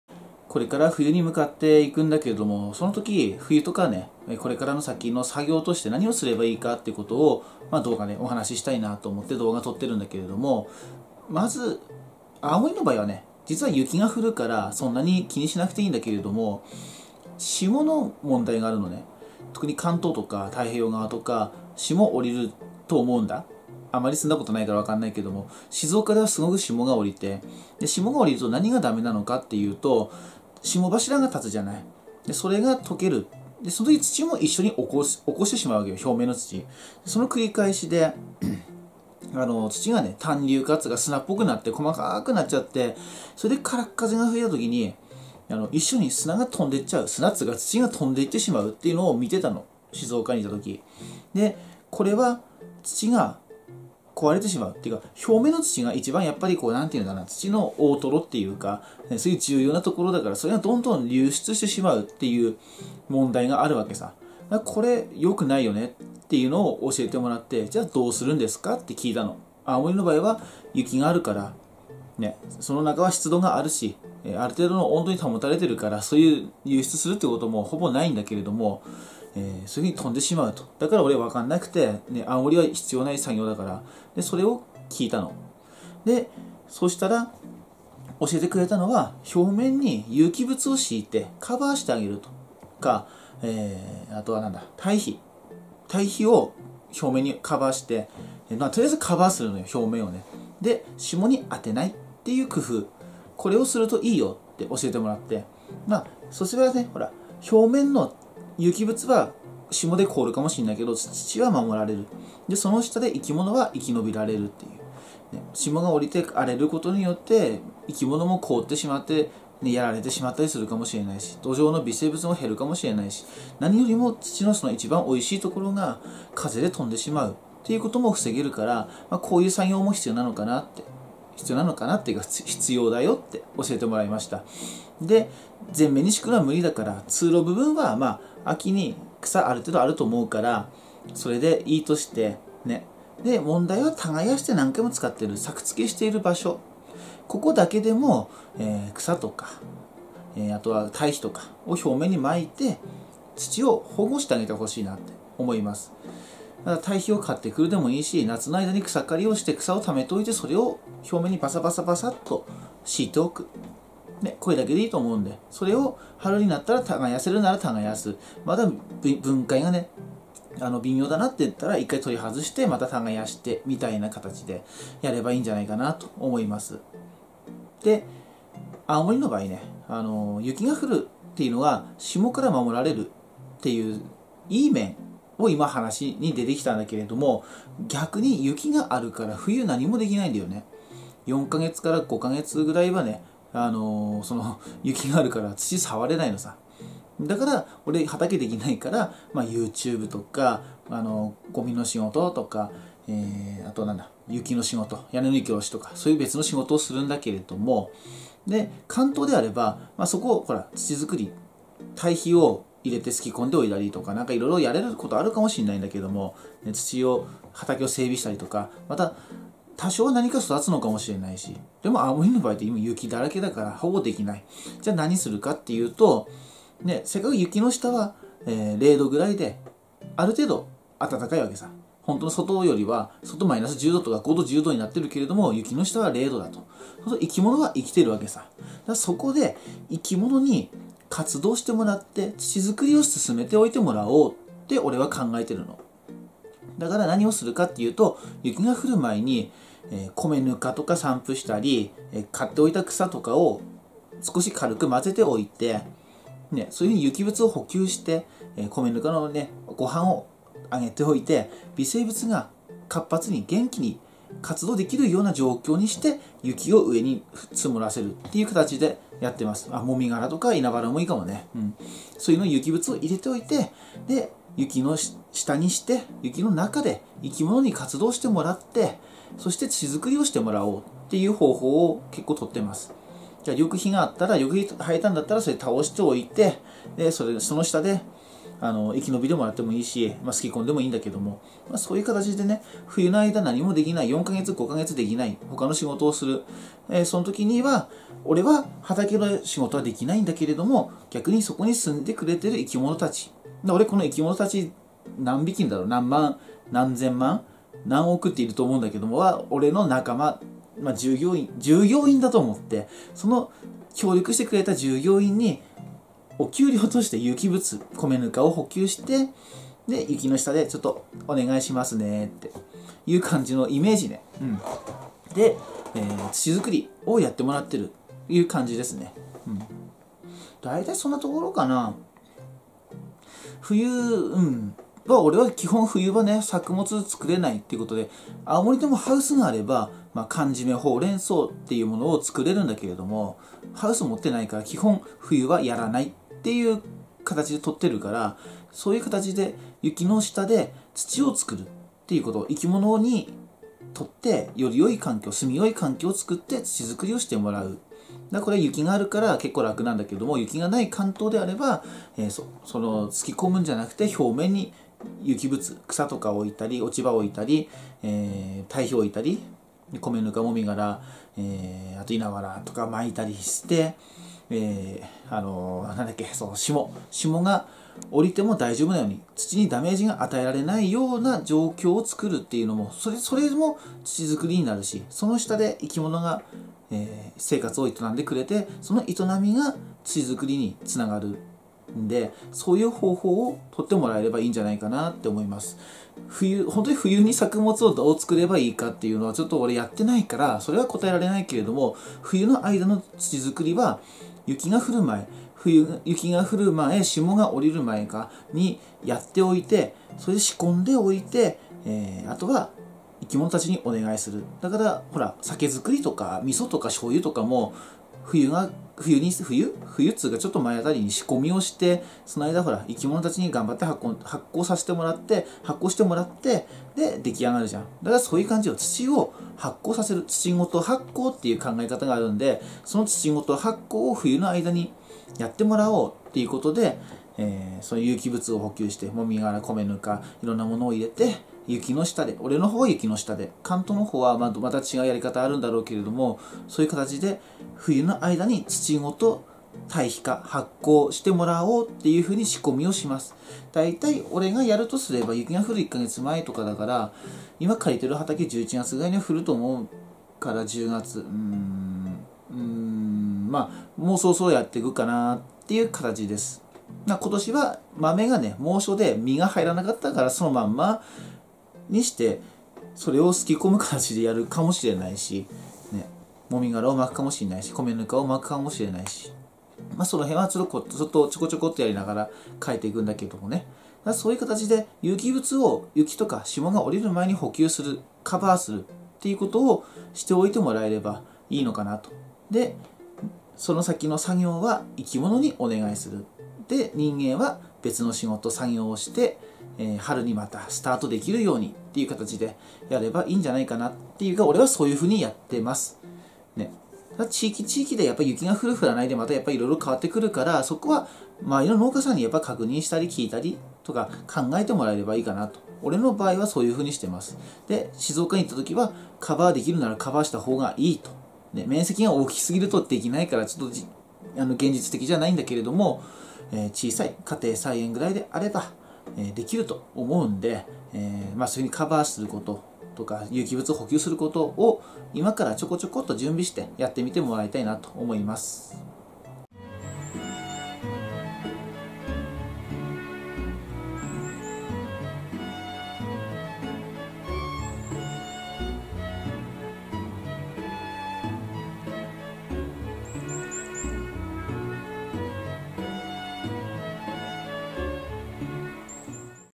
冬の前に畑ですること【土ごと発酵】【霜対策】
冬はあまり畑仕事ができないので、有機物を投入して土壌中の生き物を活性化させて土作りをしてもらってます。【土ごと発酵】ですね。
堆肥をよそで作って分解を終わったものを持ってくるんじゃなくて、畑の中でその場で堆肥を作ってしまう感じです。
これのいいところは省力化だけじゃなく、途中の発酵段階の微生物も丸ごと畑の中に取り入れることができるところです。
豊かな土壌環境を作っていきたいですね。

19歳のころから有機栽培の勉強をしています。